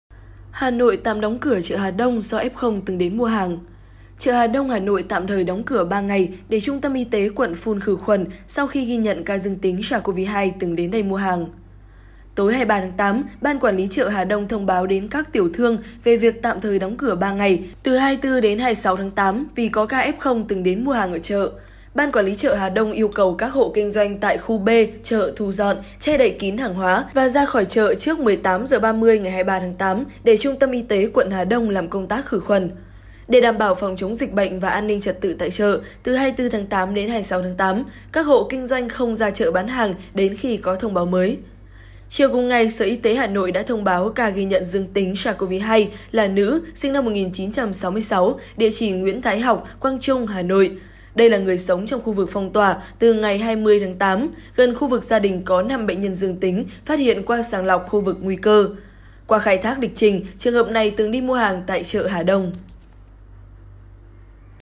Hà Nội tạm đóng cửa chợ Hà Đông do F0 từng đến mua hàng
Tối 23/8, Ban Quản lý chợ Hà Đông thông báo đến các tiểu thương về việc tạm thời đóng cửa 3 ngày (24- 26/8) vì có ca F0 từng đến mua hàng ở chợ.